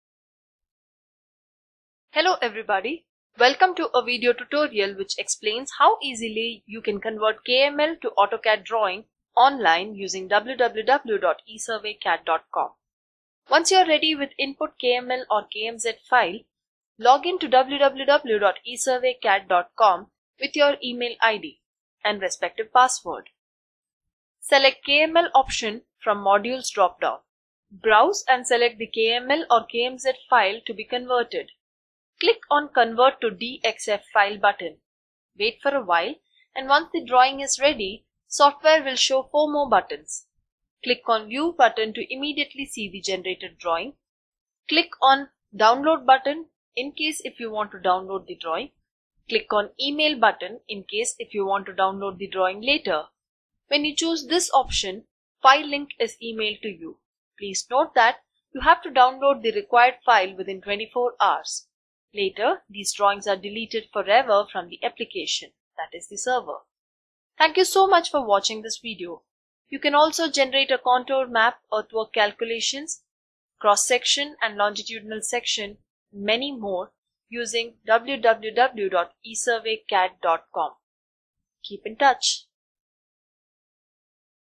Google Earth KML to Drawing File Conversion Online
a Software as a solution platform. You can easily convert the KML file as a Drawing File (DXF Format) and then open the Downloaded File in your CAD Package such as AutoCAD, BricsCAD, ZWCAD. Using this online KML Converter, Both KML and KMZ files can be converted as CAD Files.

Hello everybody, welcome to a video tutorial which explains how easily you can convert KML to AutoCAD drawing online using

Select the KML option from Modules Drop Down.

Browse and select the KML or KMZ file to be converted.  Click on the ‘Convert to DXF File’ button.

Wait for a while and once the drawing is ready, Software will show 4 more Buttons.

Click on View Button to immediately see the generated Drawing.

Click on the Download button, in case if you want to download the drawing

Click on the Email button, In case if you want to download the drawing later. When you chose this option, the file link is emailed to you. Please note that you have to download the required file within 24 hours. Later these drawings are deleted forever from the application (Server).

You can also generate a Contour Map, Earthwork Calculations, Cross Section and Longitudinal Sections many more using